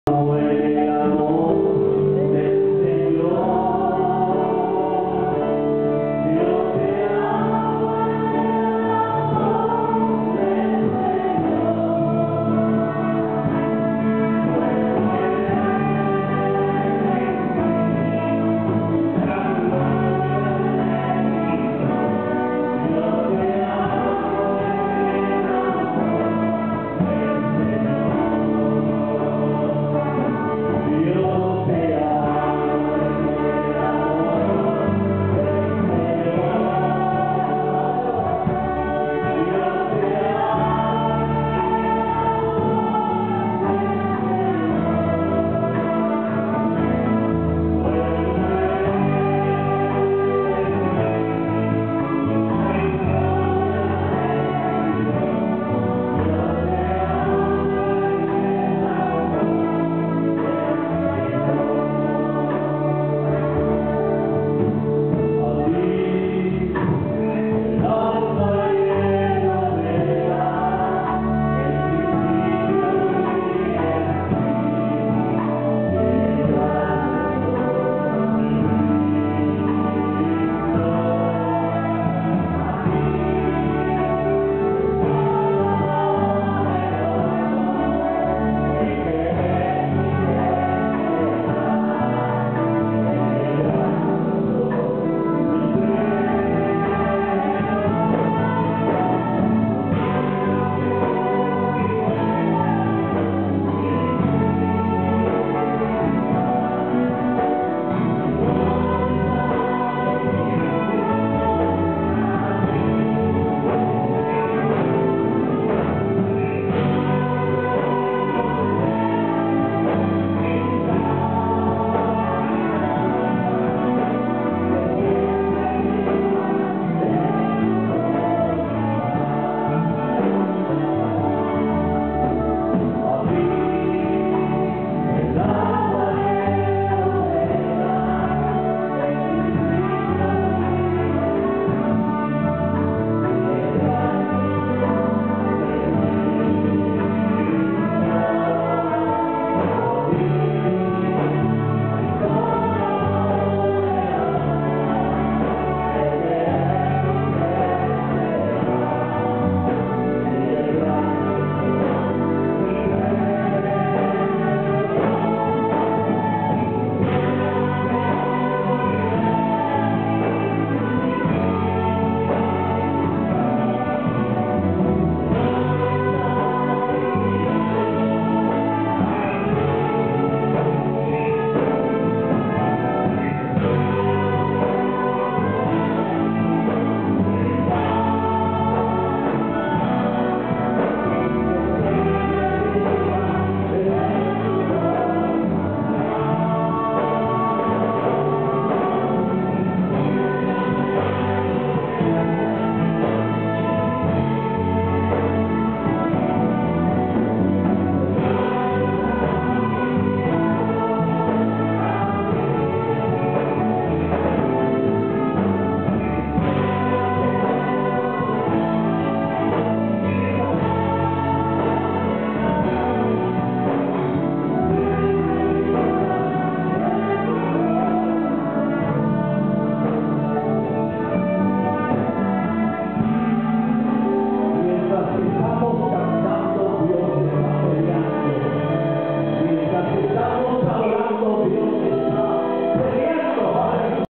Cantos de Adoracion - 12.12.10 AM
Templo Calvario Bell,
Asamblea de Dios,
Morning worship service